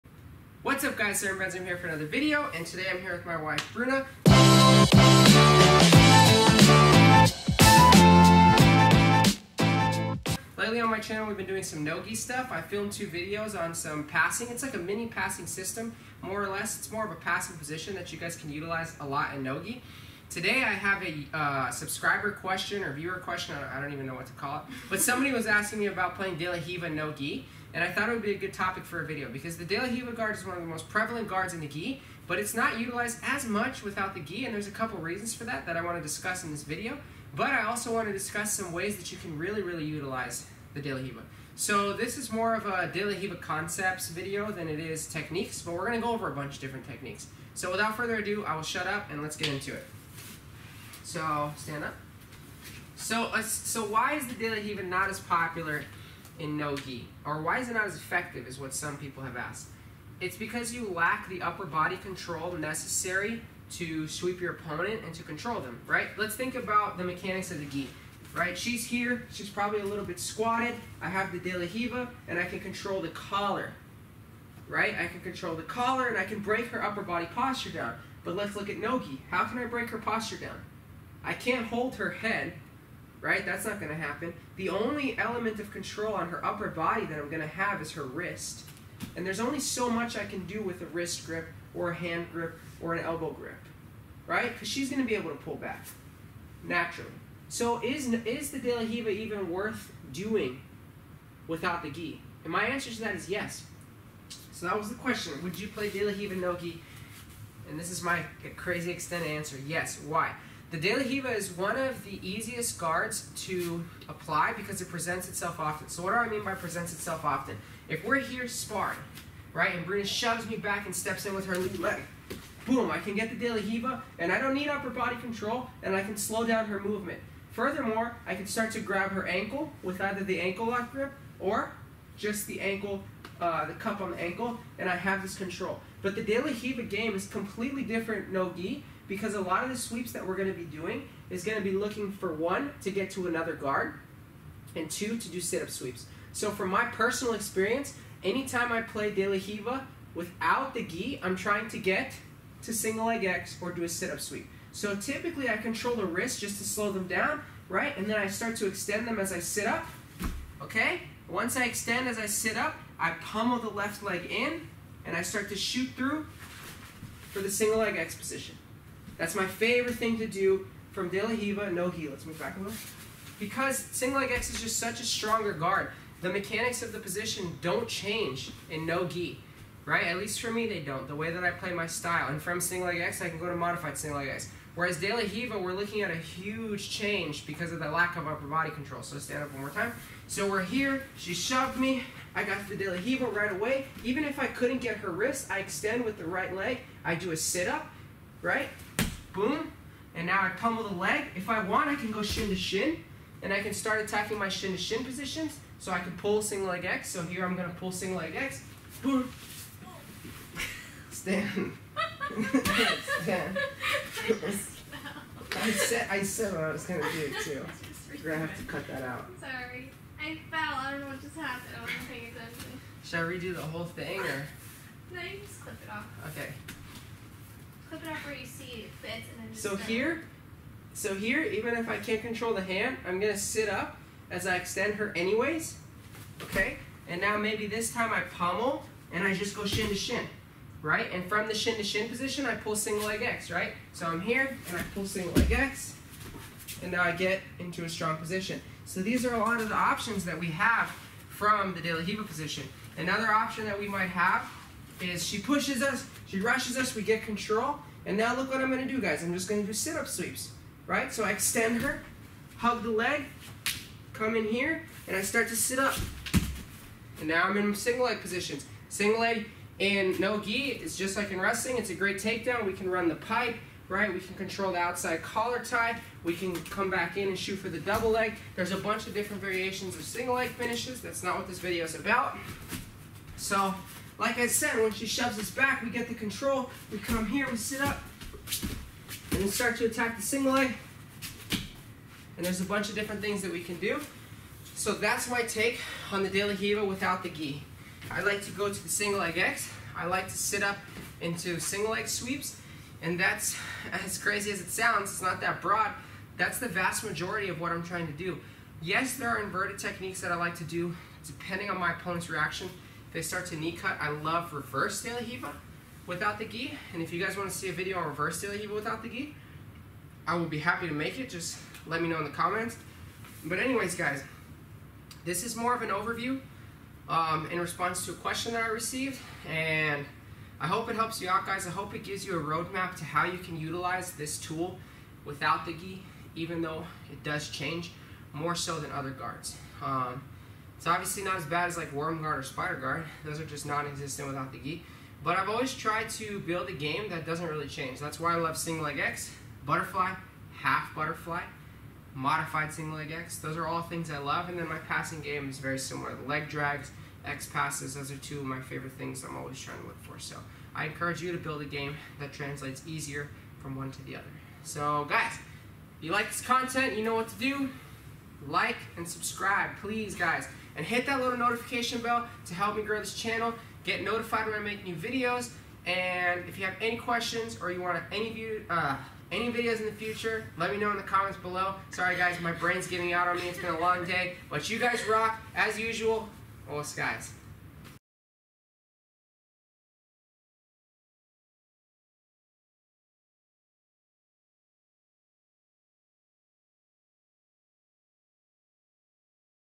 No Gi De La Riva Roadmap
NO GI DE LA RIVA ROAD MAP //

In this video I go over the concepts and principles on playing the De La Riva effectively without the gi.  A lot of people think that the De La Riva guard is not as effective without a gi but it has GREAT use.

I am a Marcelo Garcia Brown Belt, Bernardo Faria Academy Instructor and avid Brazilian Jiu Jitsu competitor. Thanks for visiting my channel! OSS!